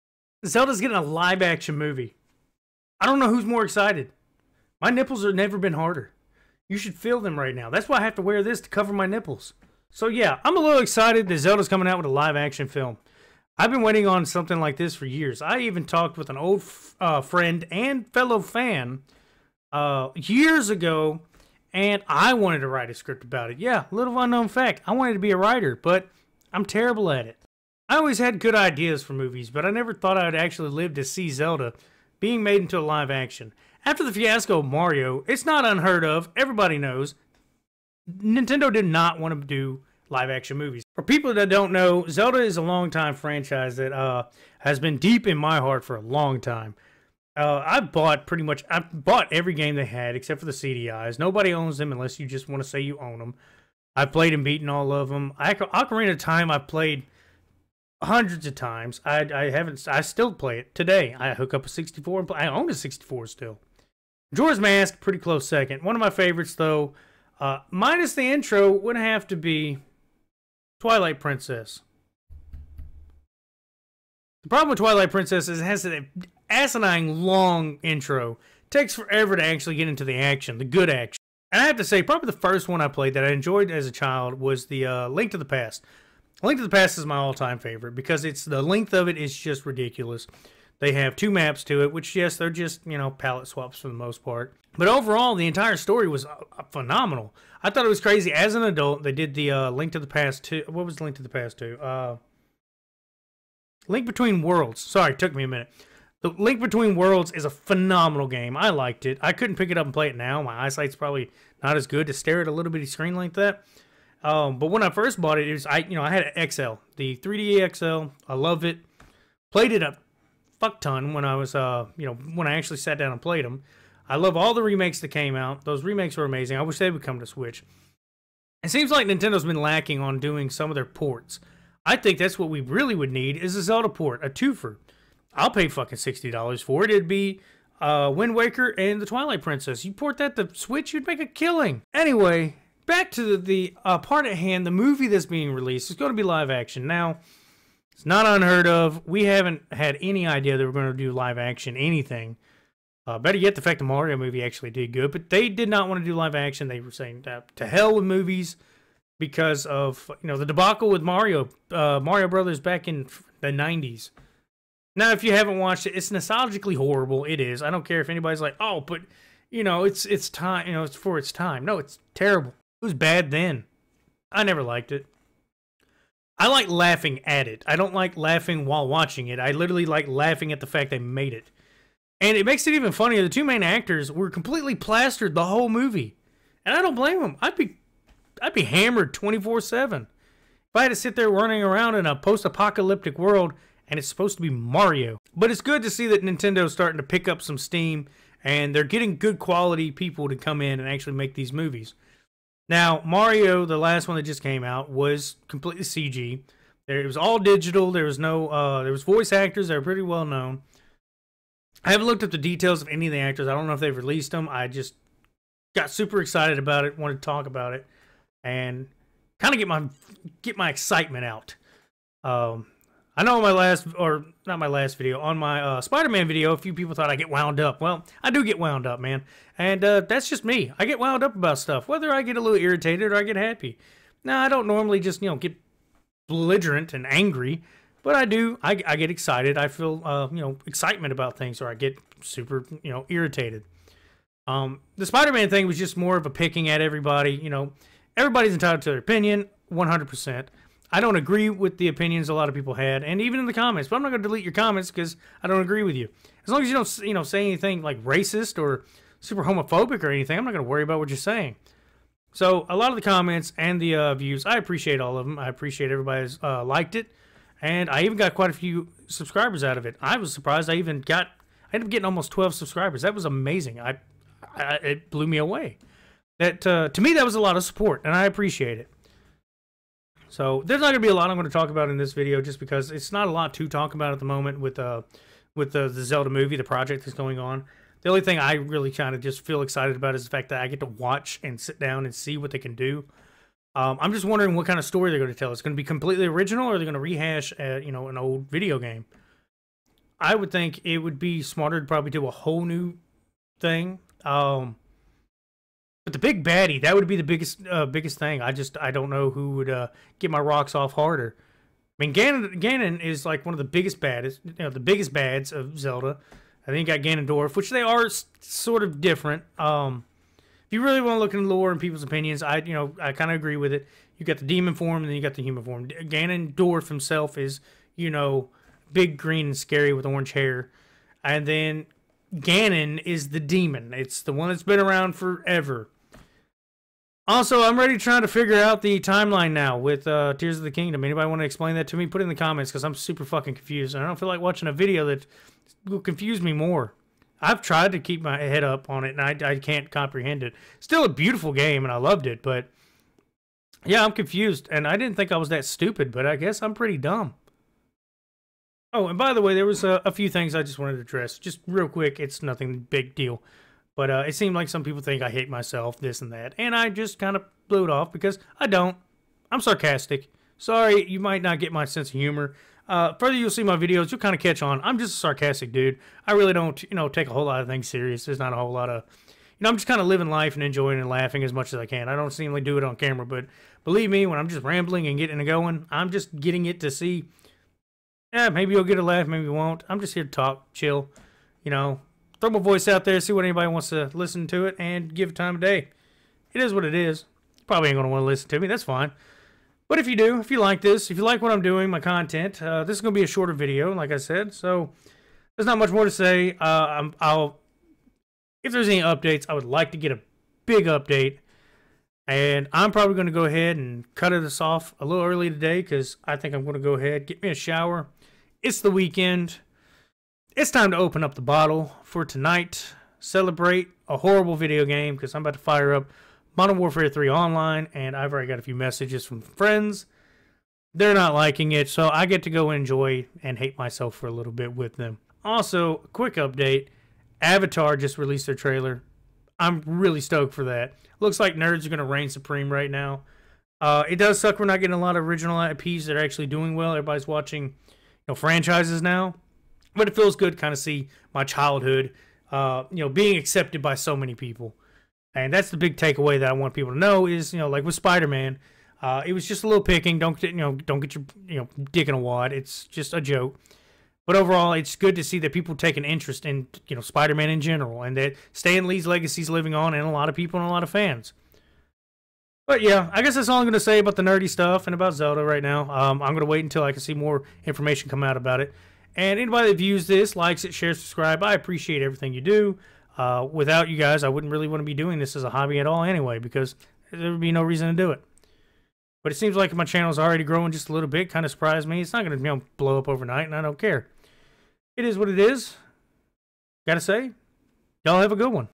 Zelda Movie Announced
Super excited for a Zelda Movie. Leave a comment on your favorite and what you would like to see adapted.